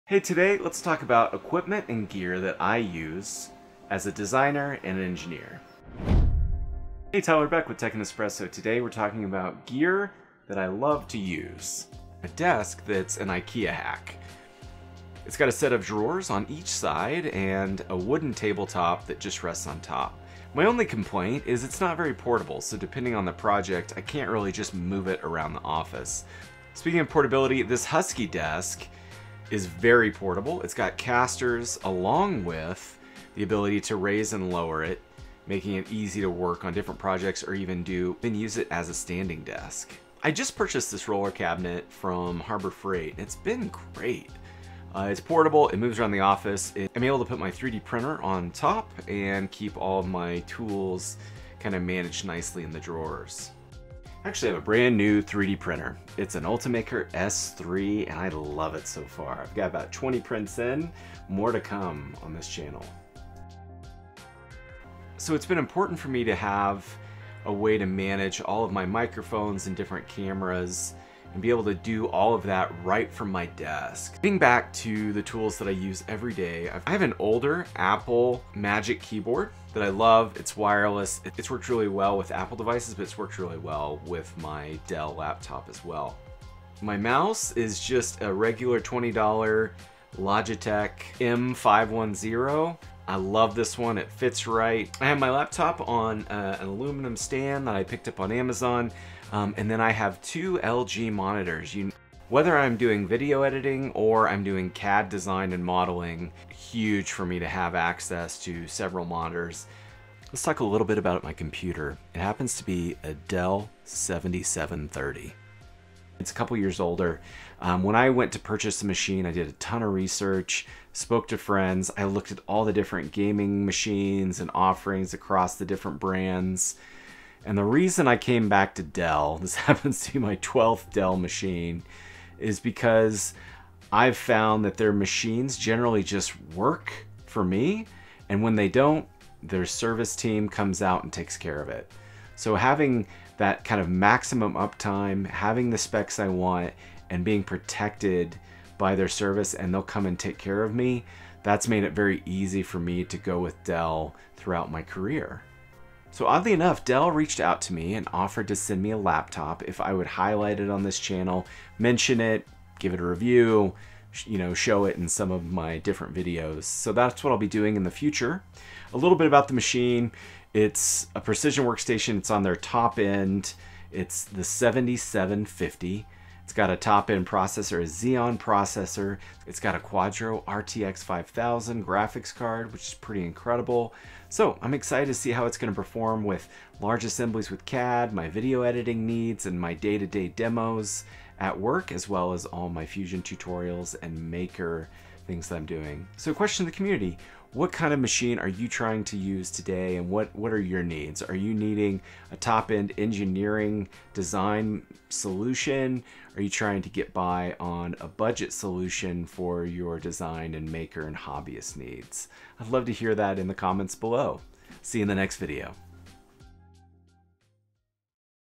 Gear I Use Everyday - Office Tour (Including My New Dell Precision 7750)
Office tour of my daily tools I use from my desk setup, 3D Printer and my computer.

Thanks Dell for gifting me this new Precision 7750 workstation! Excited to try it over the next few months!